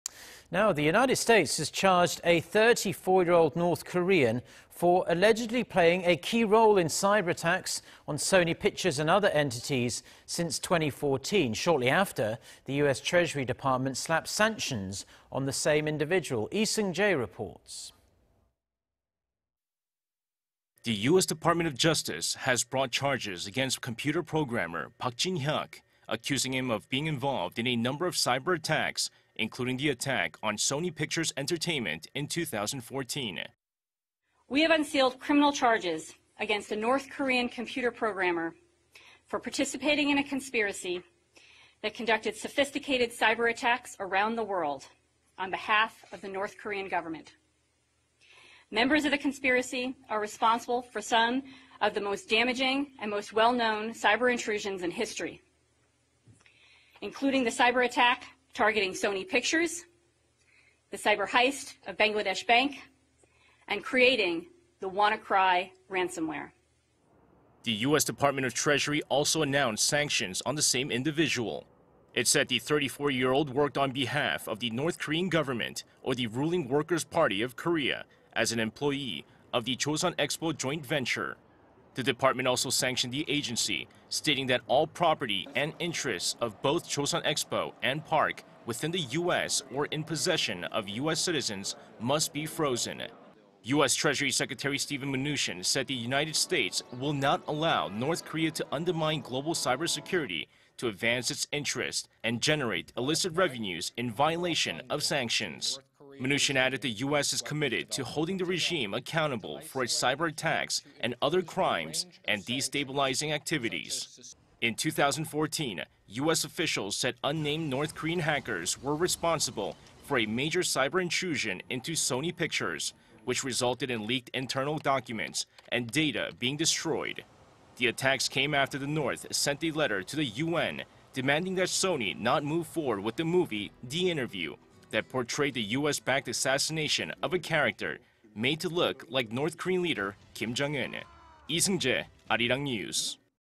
U.S. charges North Korean over cyberattacks
미 재무 "北, 글로벌 사이버보안 훼손 용납하지 않을 것"
  The United States has charged a 34-year-old North Korean for allegedly playing a key role in cyberattacks on Sony Pictures and other entities since 2014.
Shortly after,... the U.S. Treasury Department slapped sanctions on the same individual.
  The U.S. Department of Justice has brought charges against computer programmer Park Jin-hyok,... accusing him of being involved in a number of cyberattacks,... including the attack on Sony Pictures Entertainment in 2014.

  "We have unsealed criminal charges against a North Korean computer programmer for participating in a conspiracy that conducted sophisticated cyberattacks around the world on behalf of the North Korea government. Members of the conspiracy are responsible for some of the most damaging and most well known cyber intrusions in history including the cyberattack targeting Sony Pictures, the cyber heist of Bangladesh Bank and creating the WannaCry ransomware."

  The U.S. Department of Treasury also announced sanctions on the same individual. It said the 34-year-old worked on behalf of the North Korean government or the ruling Workers' Party of Korea as an employee of the Chosun Expo Joint Venture.
The department also sanctioned the agency, stating that all property and interests of both Chosun Expo and Park within the U.S. or in the possession of U.S. citizens must be frozen.
  U.S. Treasury Secretary Steven Mnuchin said the United States "will not allow North Korea to undermine global cybersecurity to advance it interests and generate illicit revenues" in violation of sanctions.
Mnuchin added the U.S. is committed to "holding the regime accountable for its cyberattacks and other crimes and destabilizing activities."
  In 2014,... U.S. officials said unnamed North Korean hackers were responsible for a major cyber intrusion into Sony Pictures,... which resulted in leaked internal documents and data being destroyed.
  The attacks came after the North sent a letter to the UN,... demanding that Sony not move forward with the movie "The Interview" that portrayed the U.S.-backed assassination of a character made to look like North Korean leader Kim Jong-un.